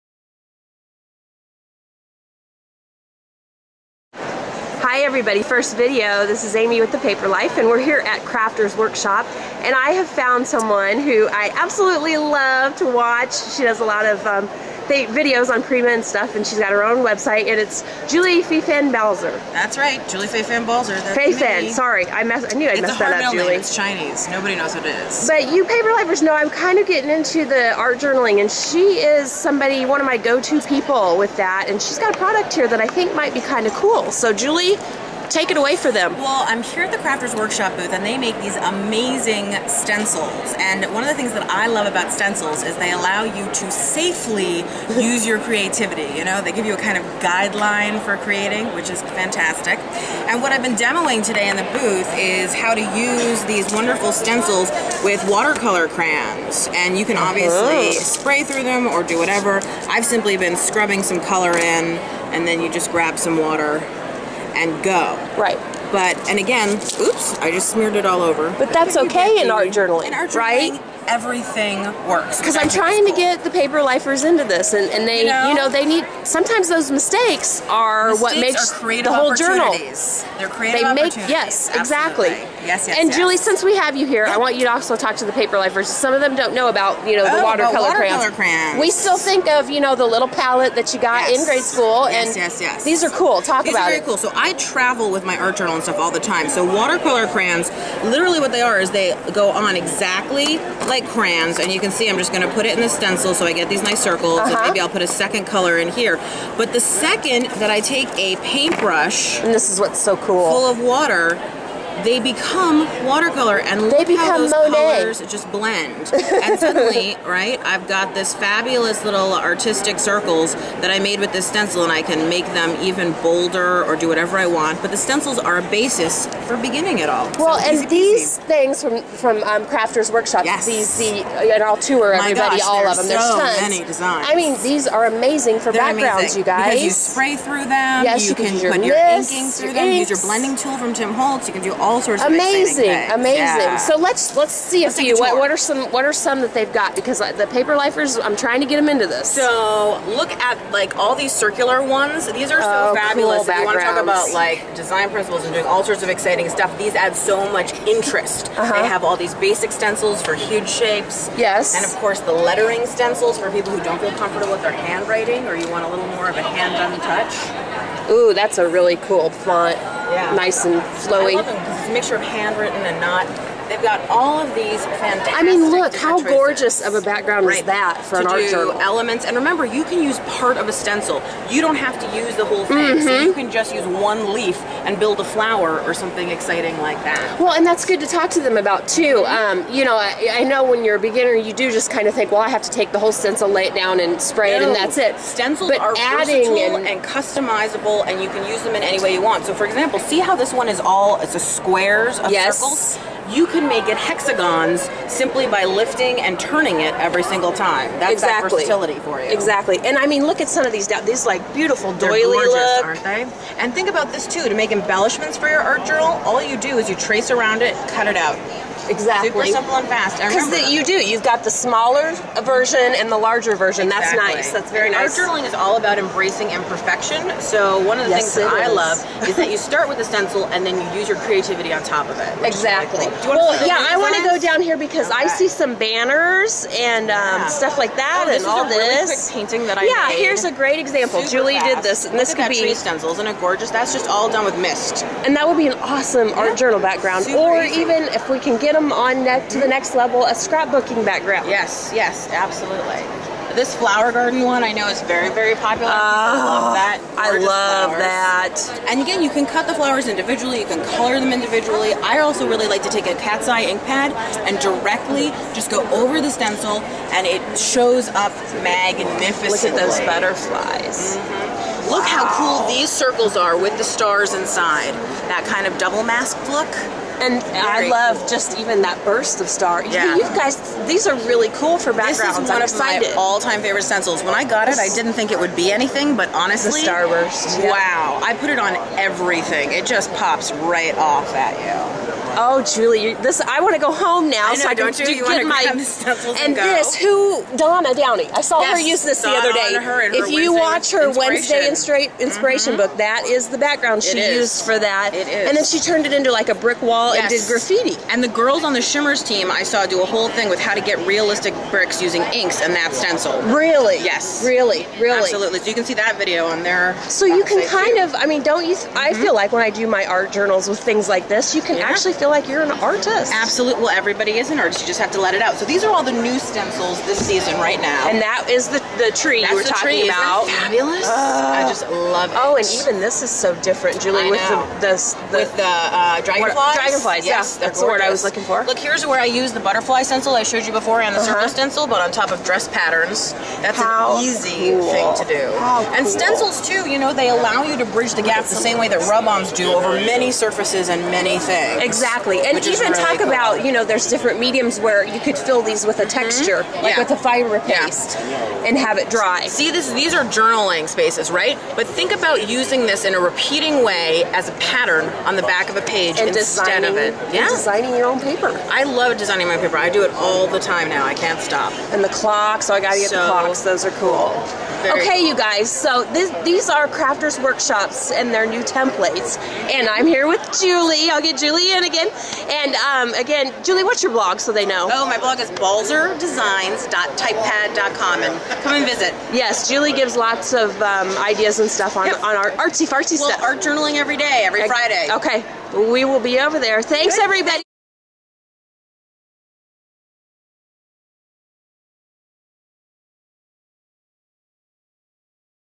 Crafters Workshop featuring julie fei fan Balzer
These stencils from Crafter's workshop are fantastic for scrapbooking and art journaling .  Watch as Julie Fei fan Baltzer gives the paperlife's Amy Coon a tour at Winter Cha 2011